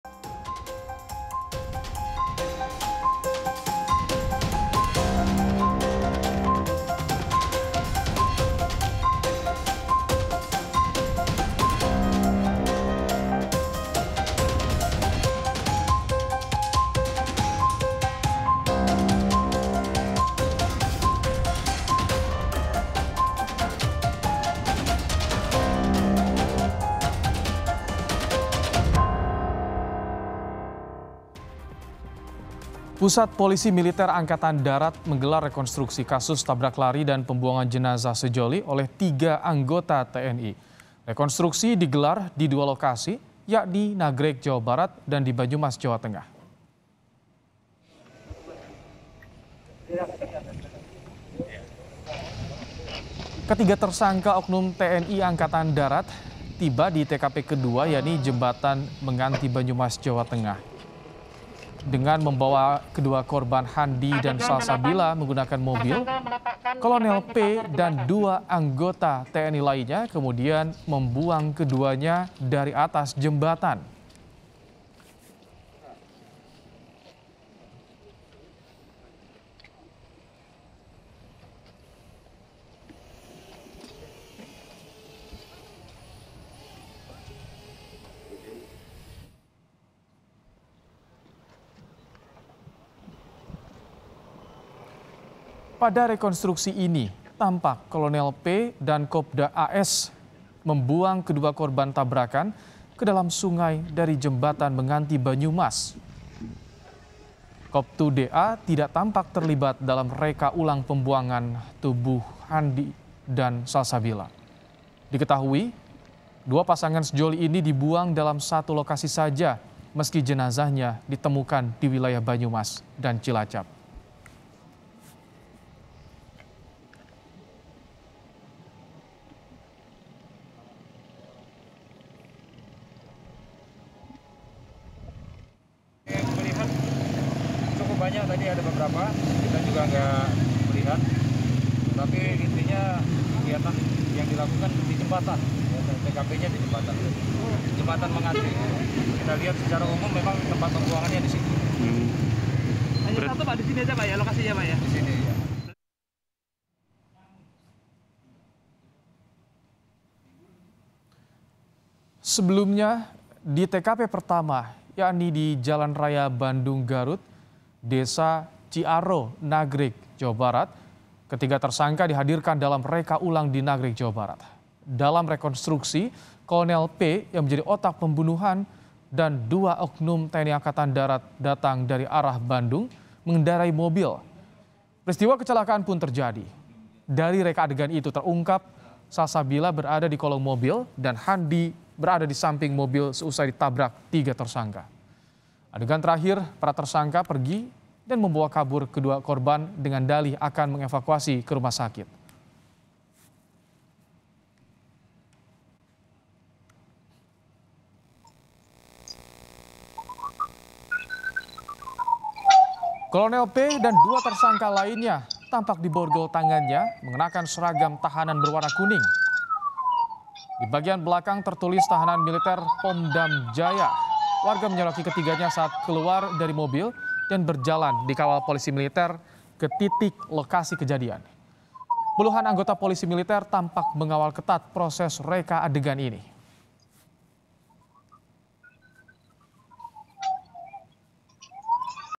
Puspomda Gelar Rekonstruksi Kasus Pembuangan Jenazah Sejoli oleh Tiga Anggota TNI
KOMPAS.TV - Pusat polisi militer angkatan darat (Puspomda) menggelar rekonstruksi kasus tabrak lari dan pembuangan jenazah sejoli oleh tiga anggota TNI.

Rekonstruksi digelar di dua lokasi yakni Nagreg, Jawa Barat dan di Banyumas, Jawa Tengah.

Ketiga tersangka oknum TNI Angkatan Darat tiba di TKP kedua yakni di Jembatan Menganti, Banyumas, Jawa Tengah.

Baca Juga Penampilan Kolonel P saat Rekonstruksi Tabrak Lari Nagreg: Kepala Digunduli hingga Tangan Diborgol di

Dengan membawa kedua korban Handi dan Salsabila menggunakan mobil, Kolonel P, dan 2 anggota TNI lainnya kemudian membuang keduanya dari atas jembatan.

Pada rekontruksi ini tampak Kolonel P dan Kopda AS membuang kedua korban tabrakan ke dalam sungai dari jembatan menganti Banyumas.

Sementara Koptu DA tidak tampak terlibat dalam reka ulang pembuangan tubuh Hendi dan Salsabila.

Diketahui, kedua pasangan sejoli ini dibuang dalam satu lokasi saja meski jenazahnya ditemukan di wilayah Banyumas dan Cilacap.

Sebelumnya, di TKP pertama yakni di Jalan Raya Bandung-Garut, Desa Ciaro, Nagreg, Jawa Barat, ketiga tersangka dihadirkan dalam reka ulang di Nagreg, Jawa Barat.

Dalam rekonstruksi Kolonel P yang menjadi otak pembunuhan dan 2 oknum TNI Angkatan Darat datang dari arah Bandung mengendarai mobil.

Peristiwa kecelakaan pun terjadi dari reka adegan itu terungkap Salsabila berada di kolong mobil dan handi berada di samping mobil seusai ditabrak tiga tersangka.

Adegan terakhir para tersangka pergi dan membawa kabur kedua korban dengan dalih akan mengevakuasi ke rumah sakit.